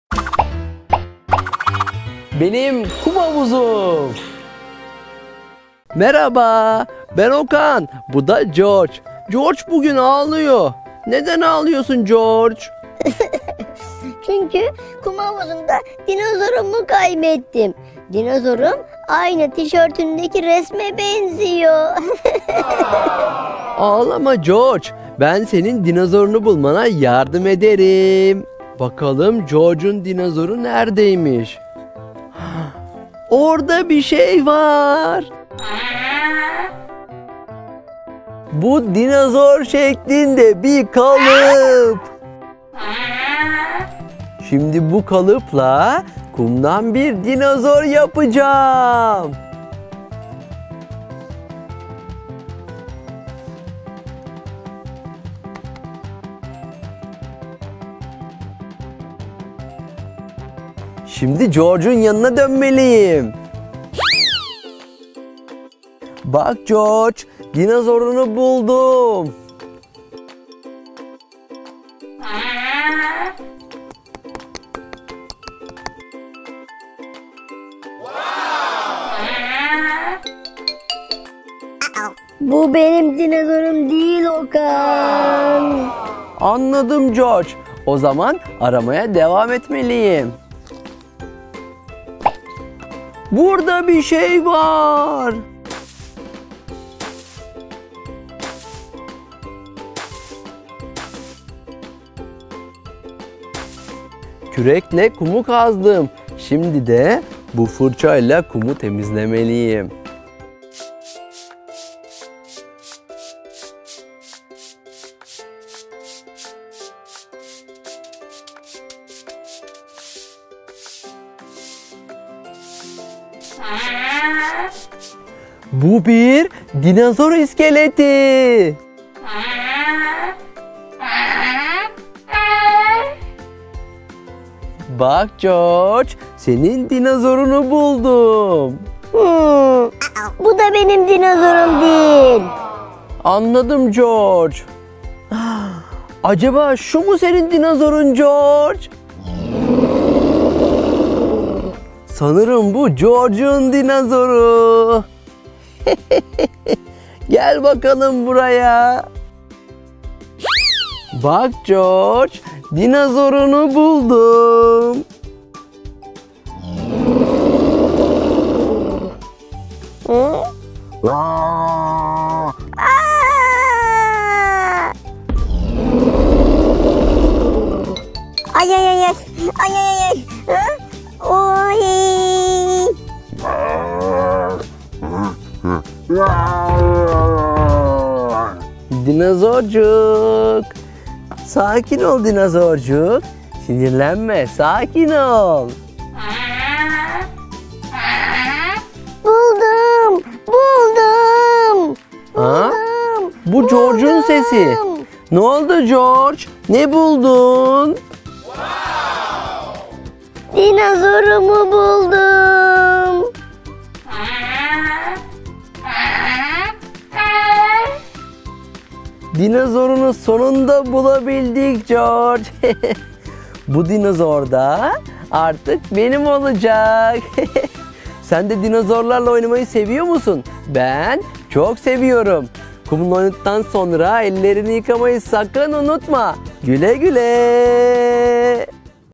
Çocuklar için kum oyunları. Domuz George için dinozor bulalım. Bebek oyunu.
Bebek oyunu. Çocuklar için kum oyunları. Okan bugün kum havuzunda oynuyor. Ah, Domuz George ağlıyor. En sevdiği dinozorunu bulamıyor. Ona yardım edelim. Veya kumdan dinozor yapalım. Bakalım, George yeni dinozoru beğenecek mi?